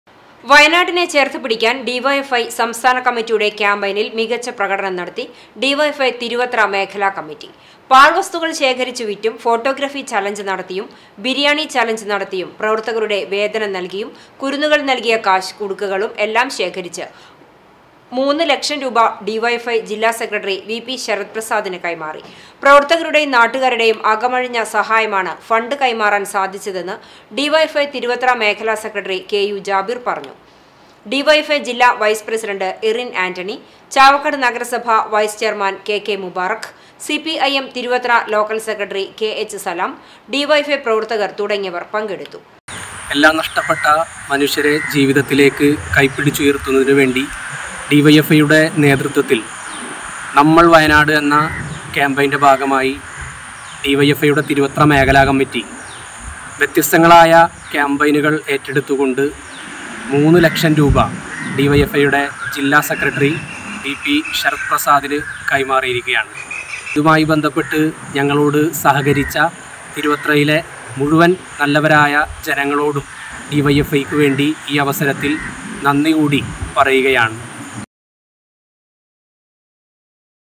വയനാടിനെ ചേര്‍ത്ത് പിടിക്കാന്‍ ഡി വൈ എഫ് ഐ തിരുവത്ര മേഖല കമ്മിറ്റി
വയനാടിനെ ചേര്‍ത്ത് പിടിക്കാന്‍ ഡി വൈ എഫ് ഐ സംസ്ഥന കമ്മിറ്റിയുടെ ക്യാമ്പയിനില്‍ മികച്ച പ്രകടനം നടത്തി ഡിവൈഎഫ്‌ഐ തിരുവത്ര മേഖല കമ്മിറ്റി.  പാഴ് വസ്തുക്കള്‍ ശേഖരിച്ച് വിറ്റും, ഫോട്ടോഗ്രാഫി ചലഞ്ച് നടത്തിയും ബിരിയാണി ചലഞ്ച് നടത്തിയും, പ്രവര്‍ത്തകരുടെ വേതനം നല്‍കിയും, കുരുന്നുകള്‍ നല്‍കിയ കാശ് കുടുക്കകളും  എല്ലാം ശേഖരിച്ച് 3 മൂന്ന് ലക്ഷം രൂപ ഡി വൈ എഫ് ഐ ജില്ലാ സെക്രട്ടറി വി.പി ശരത് പ്രസാദിന് കൈമാറി.